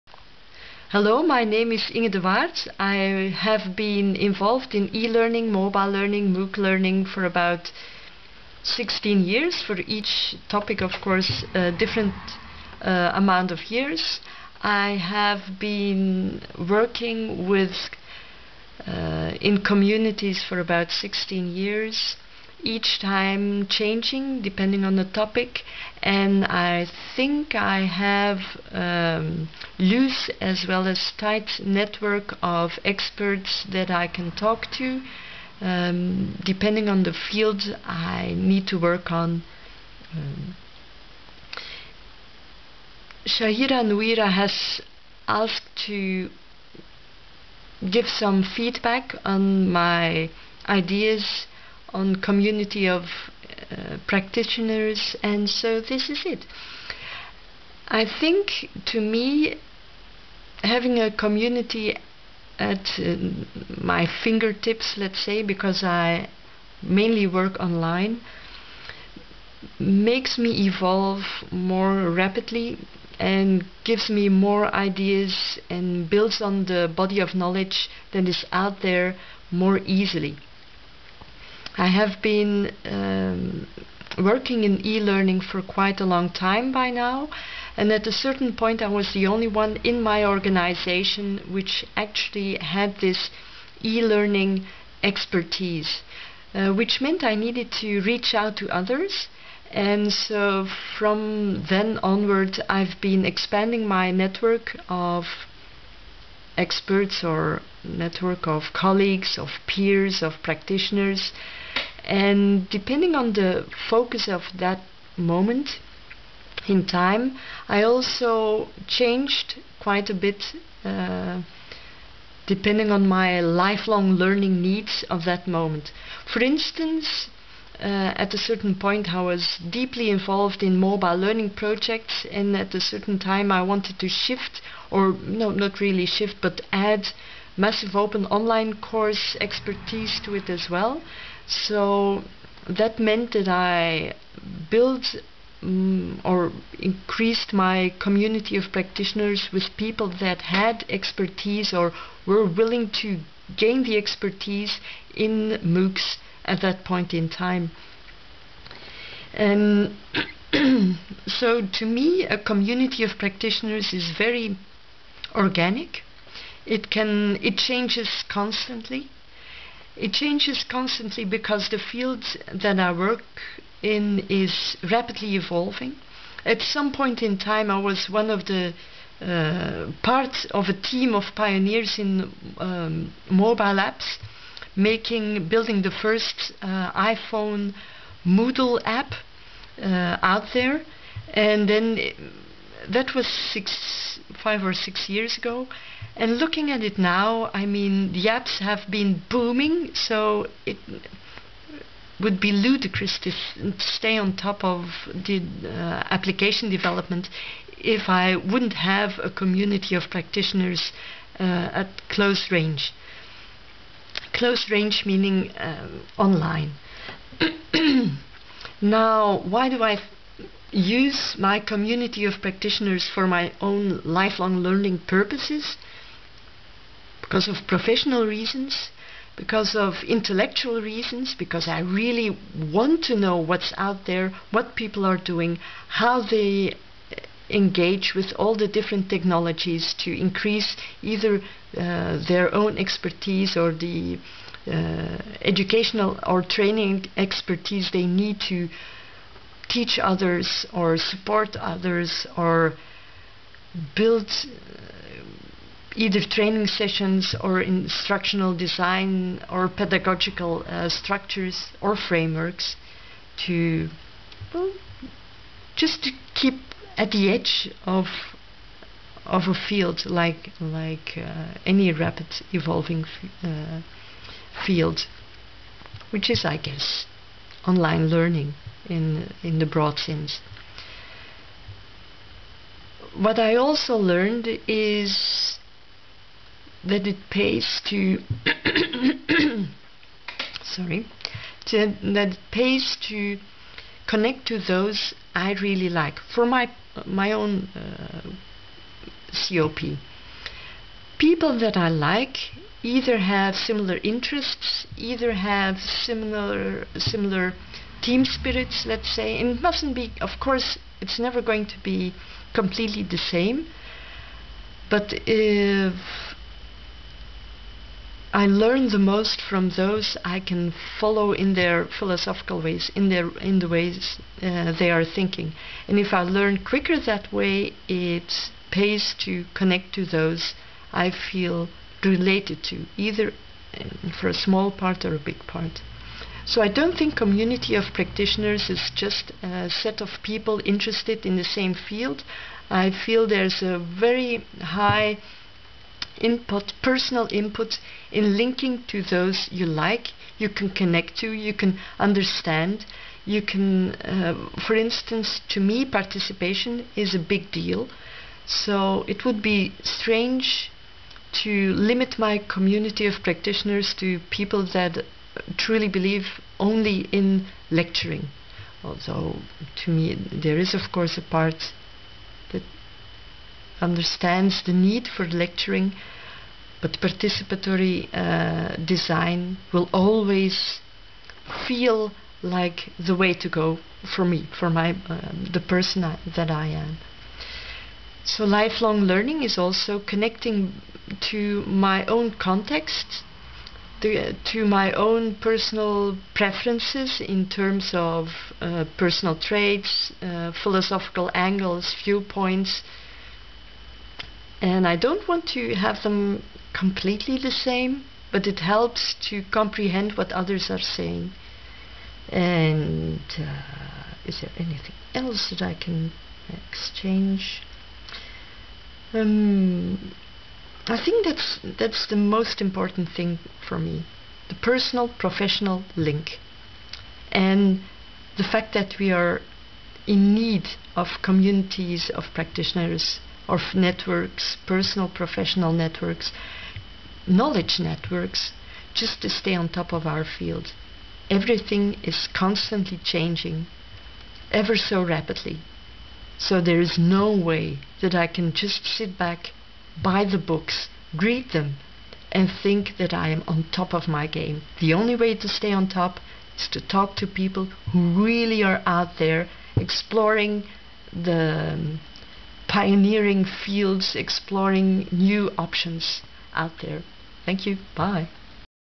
Short view on my CoP experience to support lifelong learning
Brief open share video on thoughts related to CoP and lifelong learning.